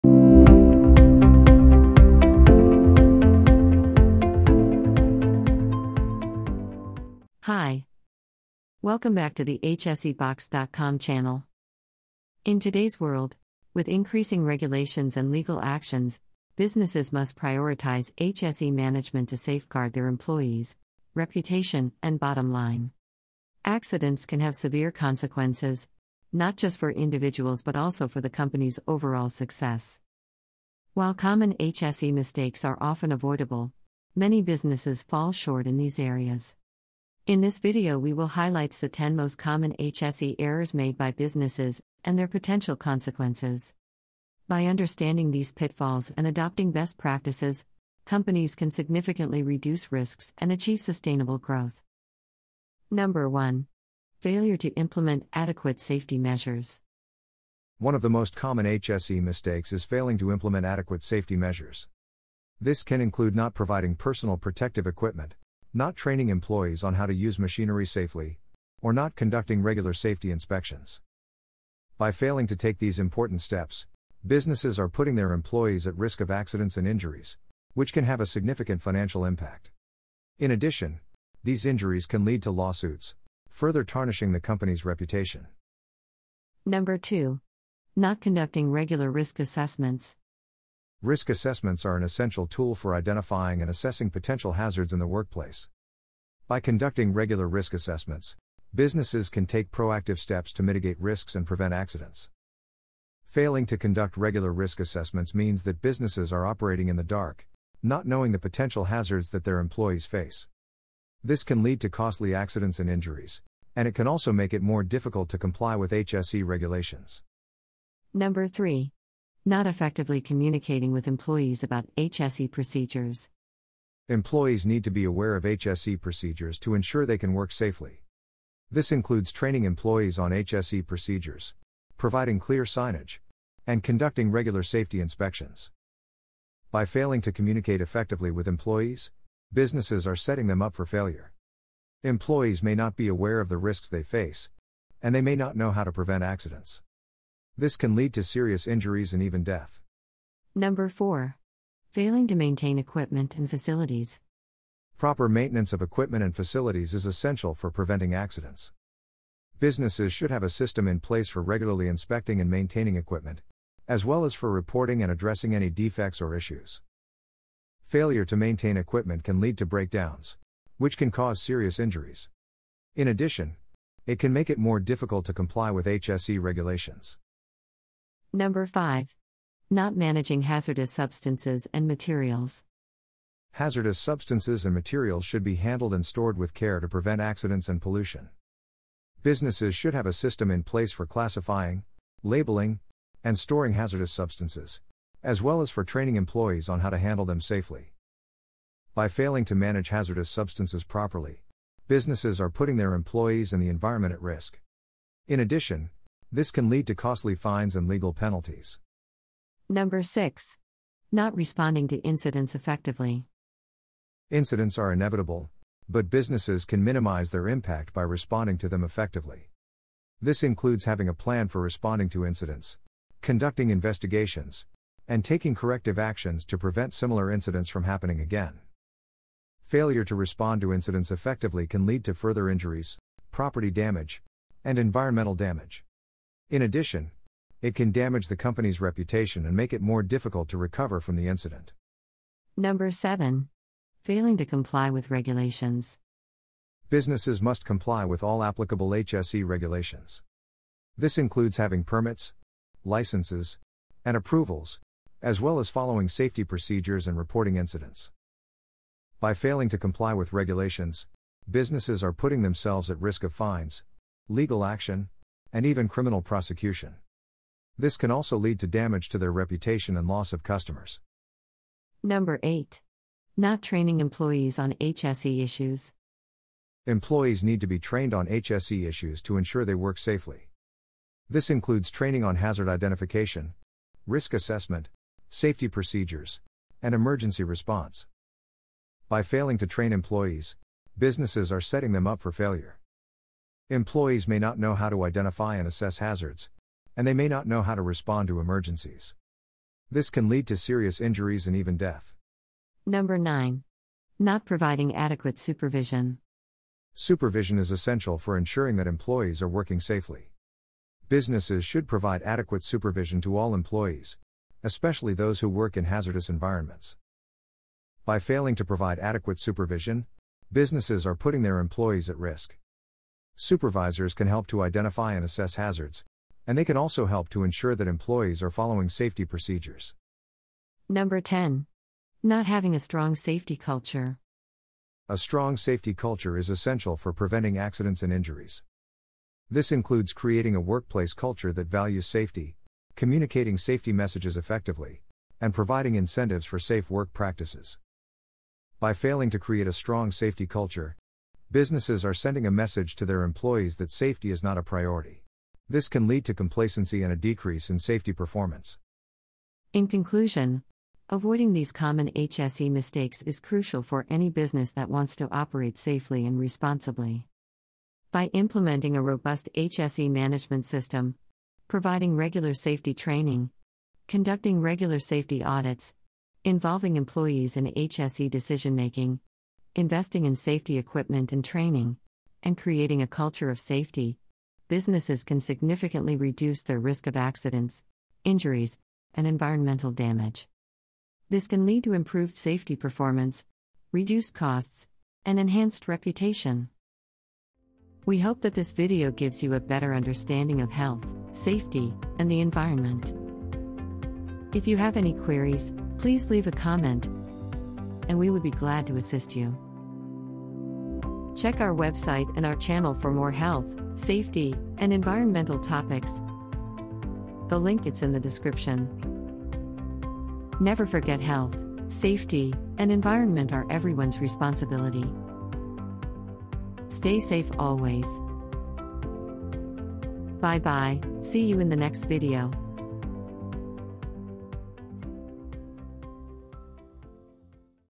10 common HSE mistakes that can cost you big - Safety Training
This video outlines 10 common HSE mistakes that businesses frequently make, which can have significant financial, reputational, and safety consequences. By understanding these pitfalls and implementing best practices, companies can significantly reduce their risk of accidents, injuries, and environmental damage.